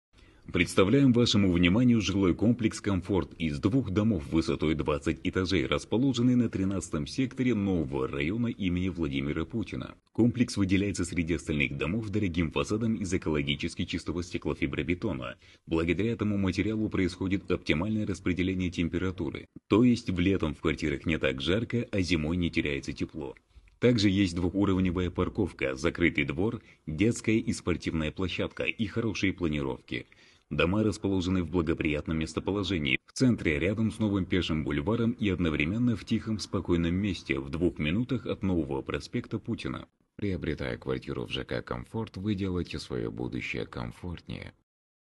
СТАРТ ПРОДАЖ НОВОГО ЖК «КОМФОРТ»
Впервые! Отсрочка на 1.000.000₽ до 2027 года! Такого еще ни делал ни один застройщик!
Уважаемые друзья впервые из застройщиков мы презентуем отсрочку платежа до 01.2027 г в размере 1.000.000 при взносе от 500.000₽.

К примеру вы покупаете квартиру с 16 по 20 эт 38.9 кв.м в рассрочку на 4 года со взносом в размере 500.000₽.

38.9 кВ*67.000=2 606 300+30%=
=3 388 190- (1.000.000 отсрочка)=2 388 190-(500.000 взнос)=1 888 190/48=39.337 (ежемесячный платеж на 48 мес)